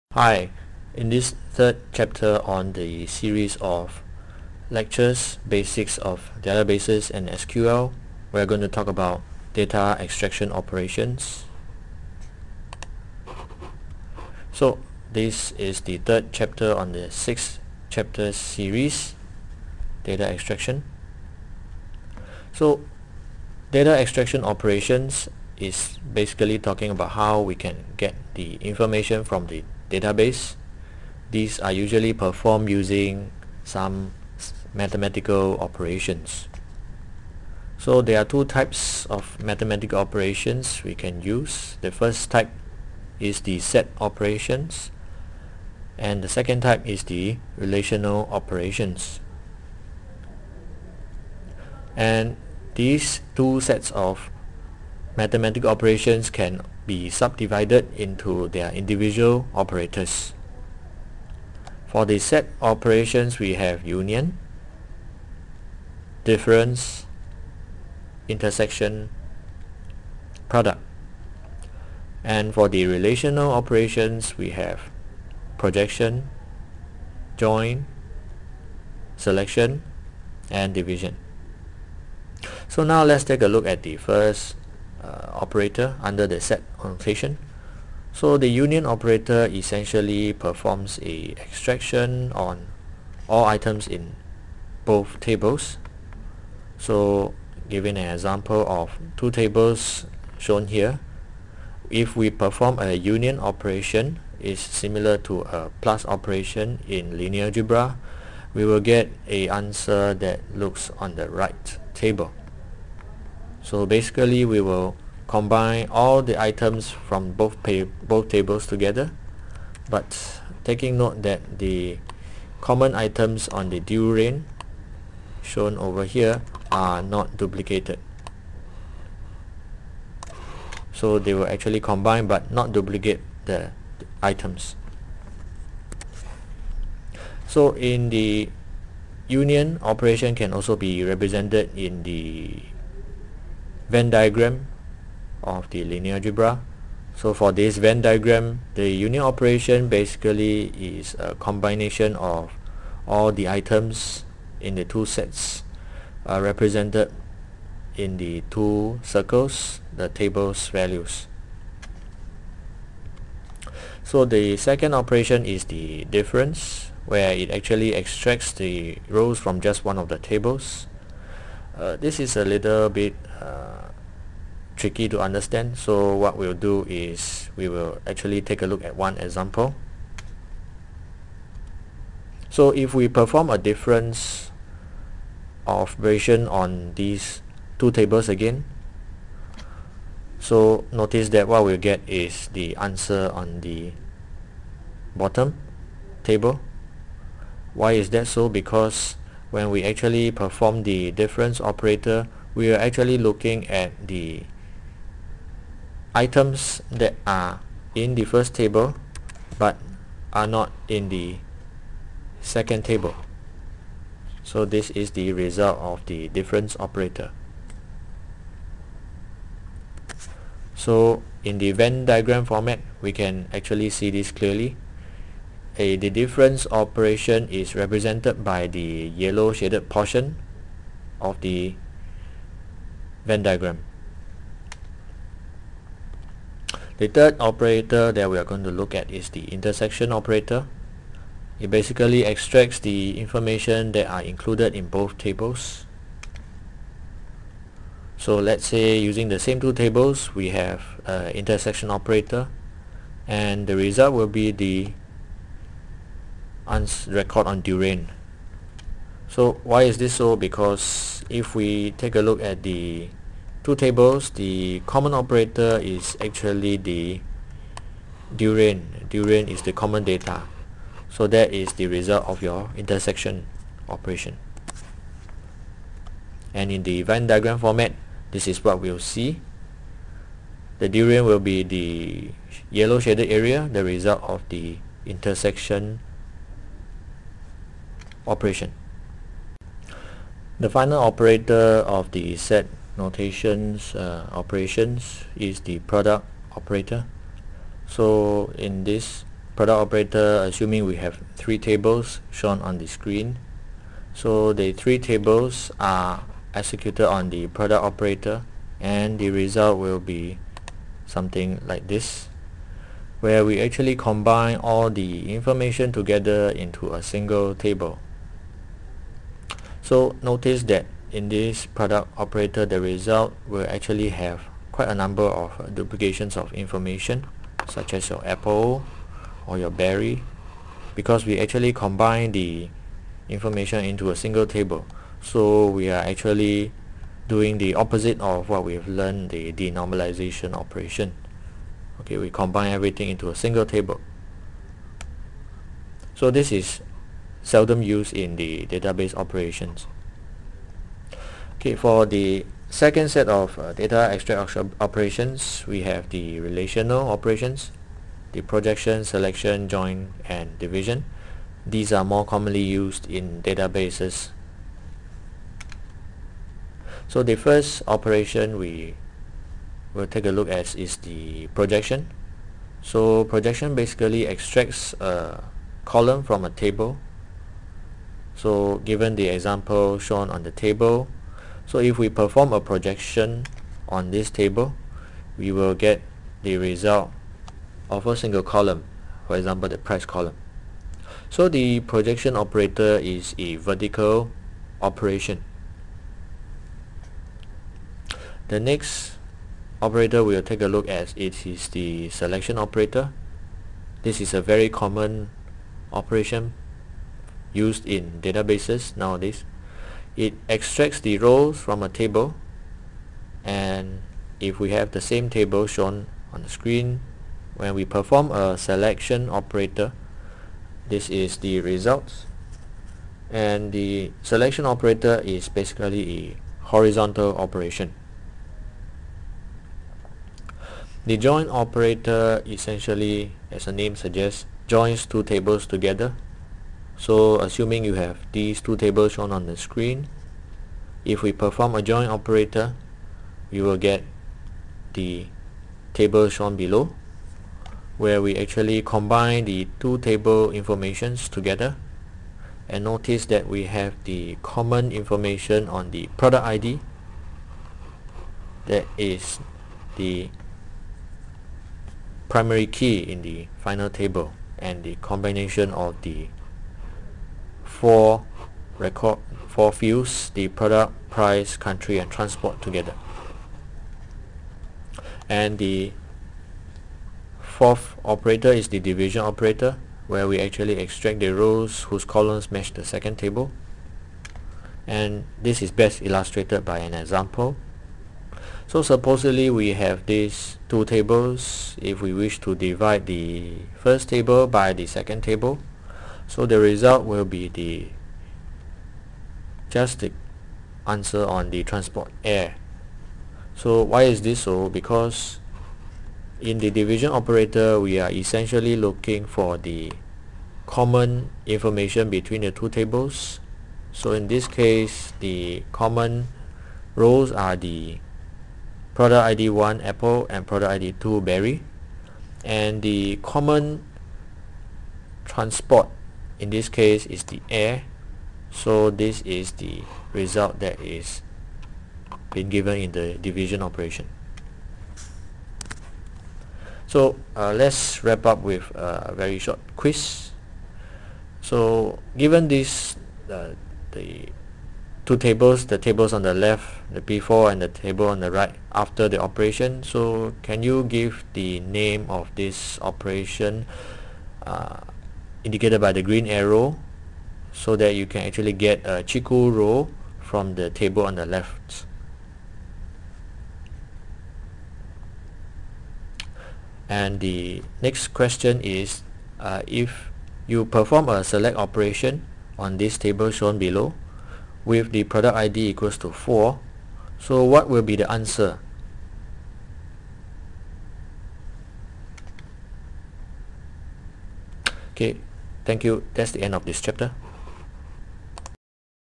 Chapter 3 - Data Extraction Operations
This is a series of videos that talks about the basics of databases and SQL.

Chapter 3 covers the following:
- Types of data extraction operations
- Set operators (Union, Difference, Intersection, Product)
- Relational operators (Projection, Join, Selection, Division)